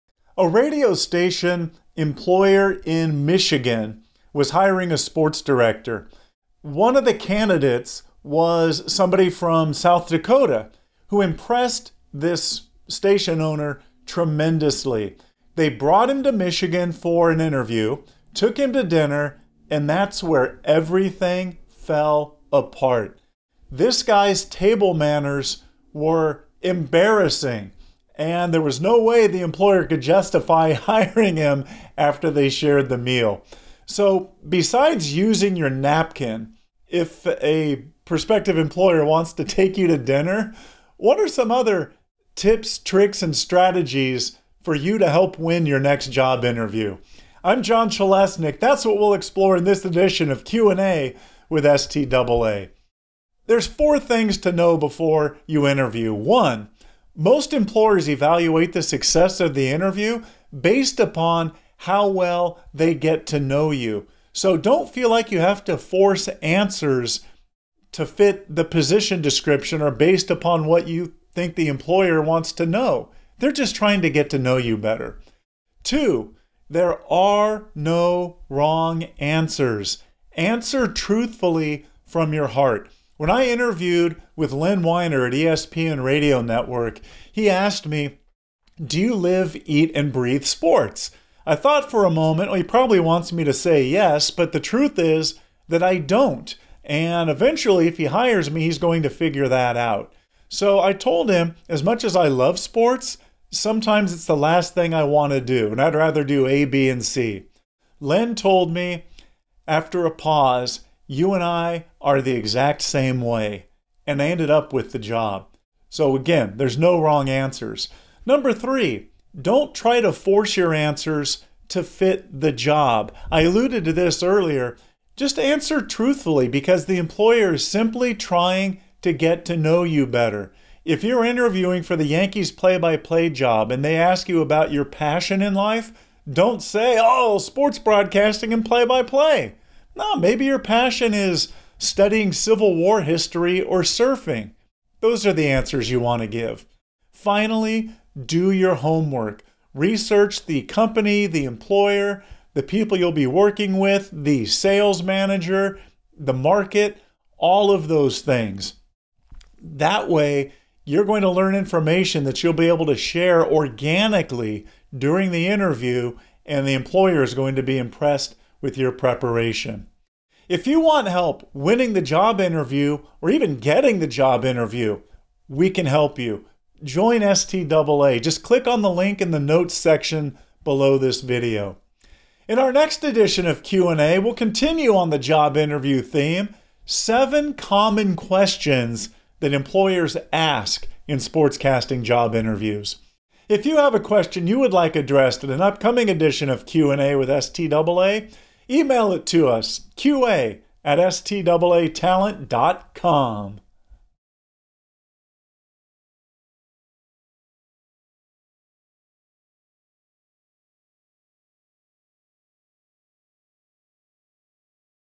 4 Tips For WINNING Your Job Interview
Job interviews can be nerve wracking, but they don't have to be. Here are 4 tips to getting ready to win your next job interview.